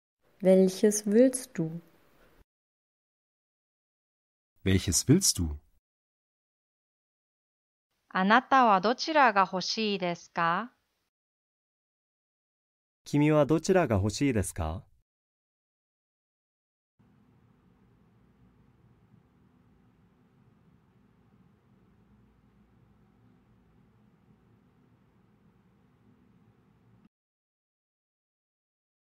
ドイツ語で"君はどちらが欲しいですか？"の発音の仕方
男性コーチ
Welches willst du?

女性コーチ
Welches willst du?

君はどちらが欲しいですか？

あなたはどちらが欲しいですか？